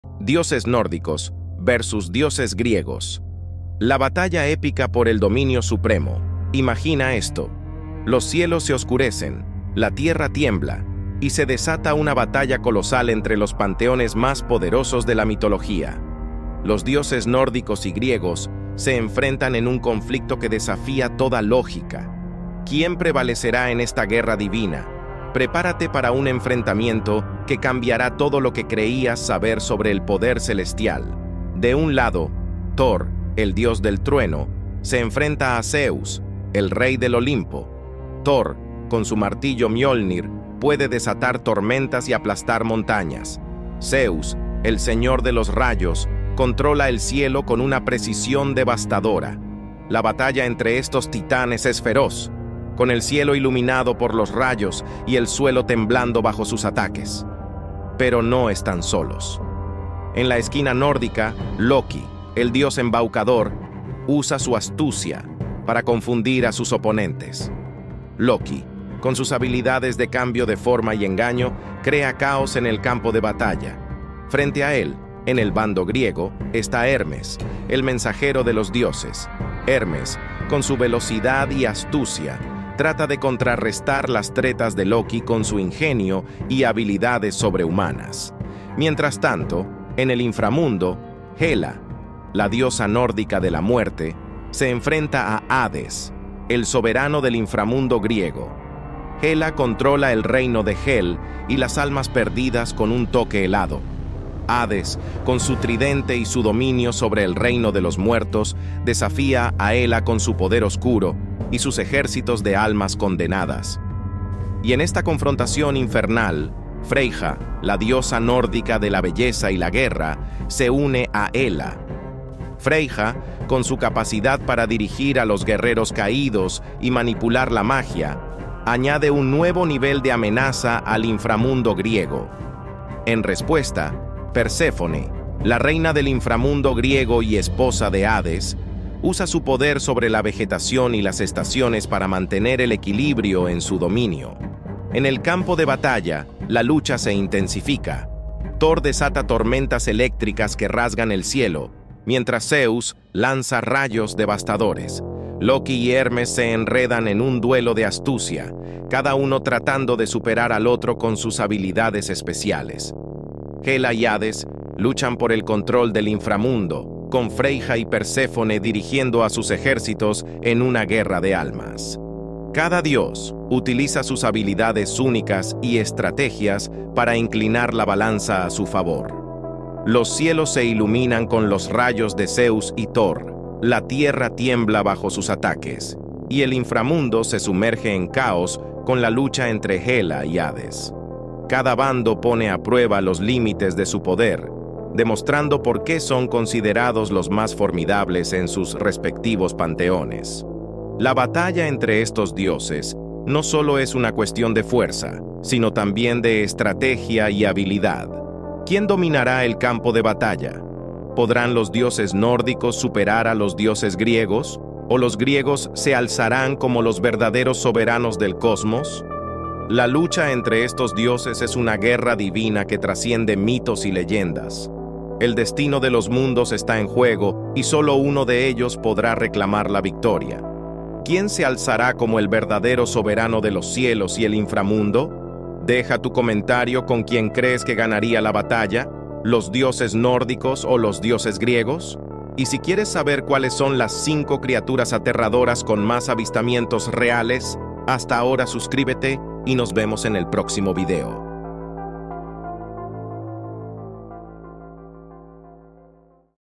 El COMBATE EPICO DE LOS DIOSES .THOR VS ZEUS
"¿Quién saldría victorioso en una épica batalla entre los dioses nórdicos y los dioses griegos? En este video, exploramos las habilidades, poderes y mitos que rodean a figuras legendarias como Zeus, Thor, Odin y Hades. ¿Qué panteón tiene la ventaja en esta confrontación de titanes? ¡Descúbrelo aquí!"